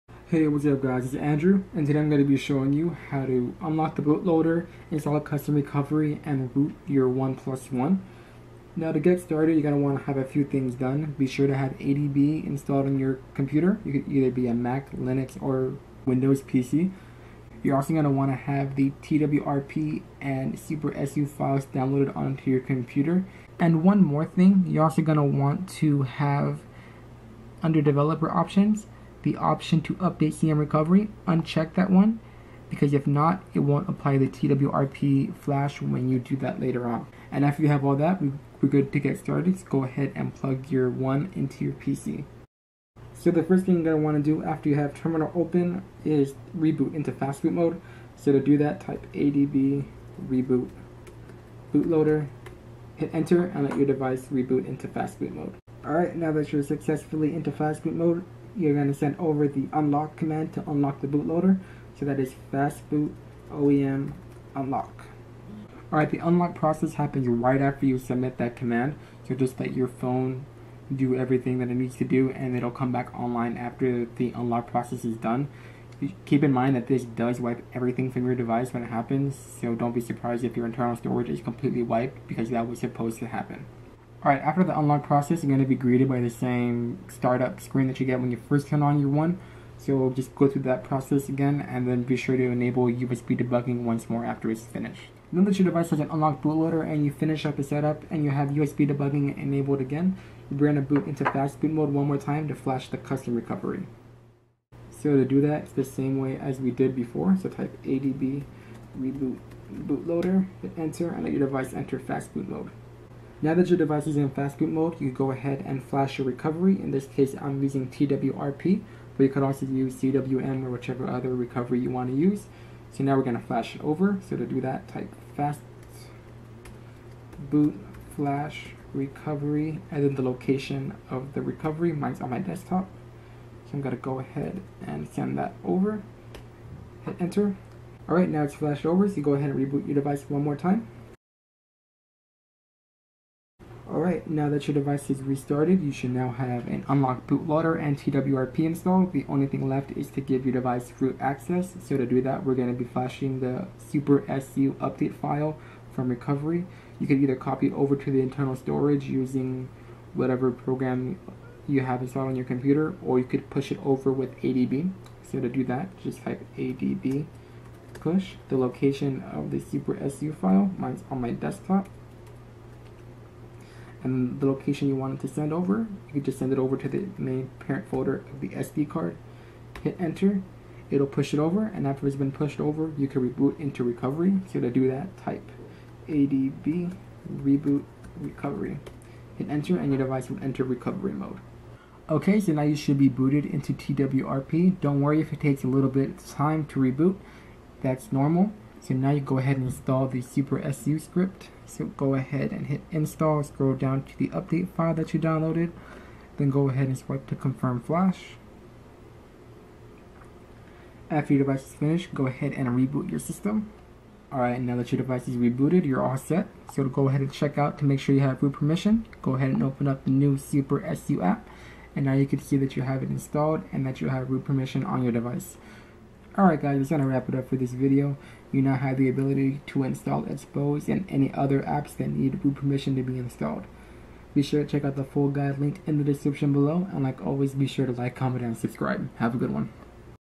Unlock Bootloader, Install Custom Recovery, & Root - OnePlus One [How-To]
How to Root the OnePlus One Phone

In this tutorial, I'll show you how to root your OnePlus One. To do this, we will first need to unlock the bootloader and install a custom recovery. This process is just as easy as it is on a Nexus device, so all you will need is a computer with ADB installed and you will be all set.

It should take no more than 10 minutes to get everything up and running. Keep in mind that this will delete everything on your device in the process, so backing up is a must. I will be using TWRP as my custom recovery, but you can also use CWM if you like.